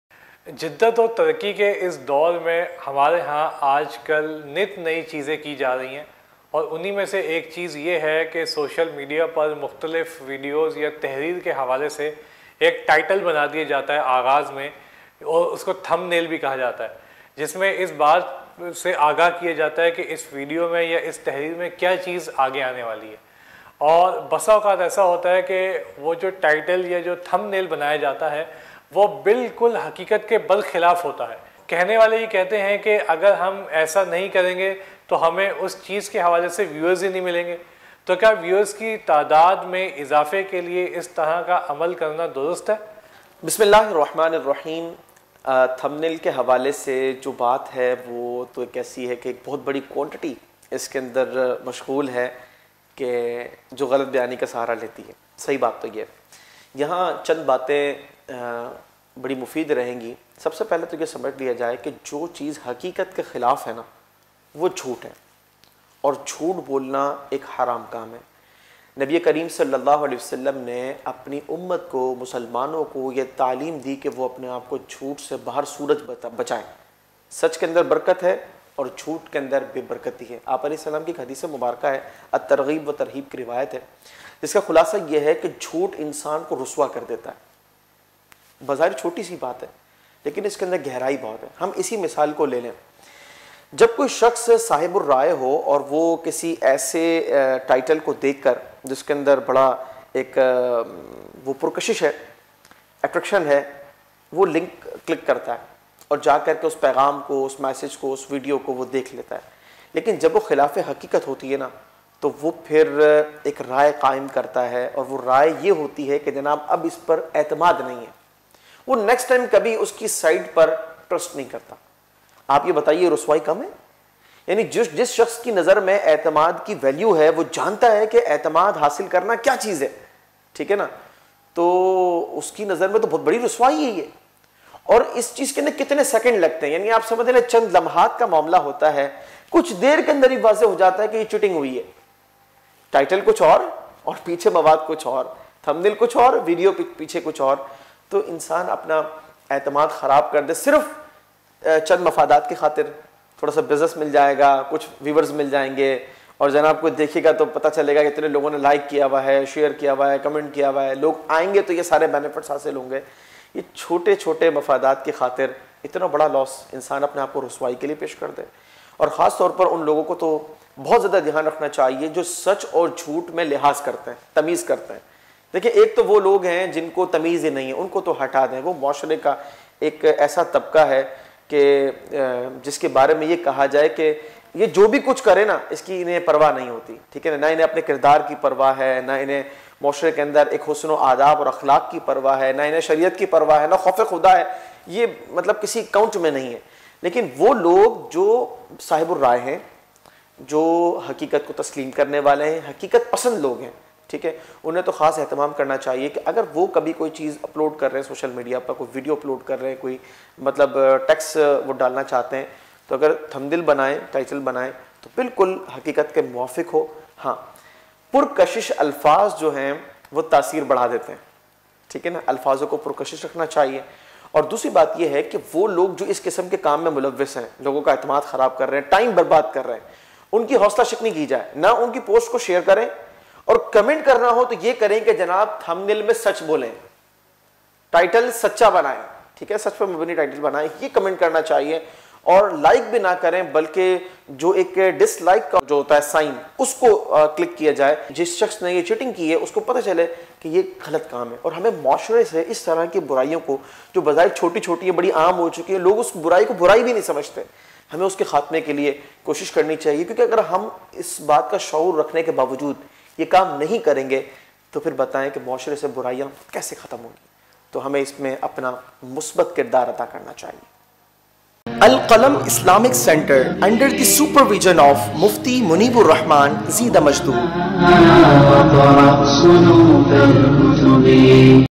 Jhoote Thumbnail
Haqiqat K Khilaf Dikha Kar Kisi Cheez Ki Taraf Raghib Karna Kesa Hai???
حقیقت کے خلاف دکھا کر کسی چیز کی طرف راغب کرنا کیسا ہے؟؟؟